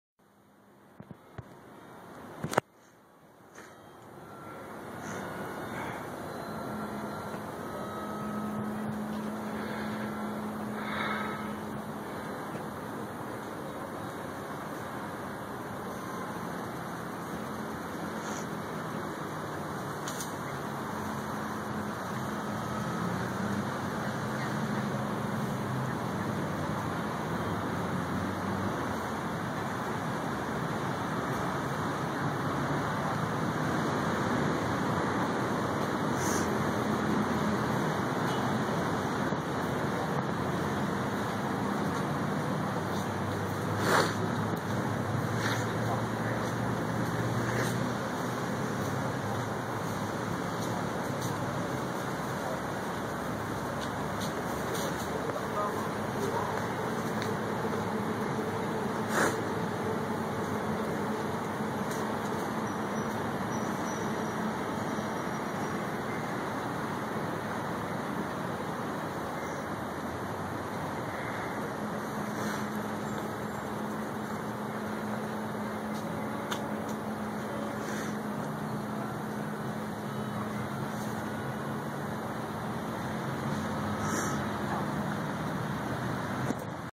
2 Monitor Lizard fighting @ Jurong Lake Park (Part 1)(Rotated + Stablized) HD
Spotted 2 monitor lizard fighting or hugging XD @ Jurong Lake Park while cycling.
In the background still can hear ppl playing violin lol

Video from my Motorola Atrix 2 on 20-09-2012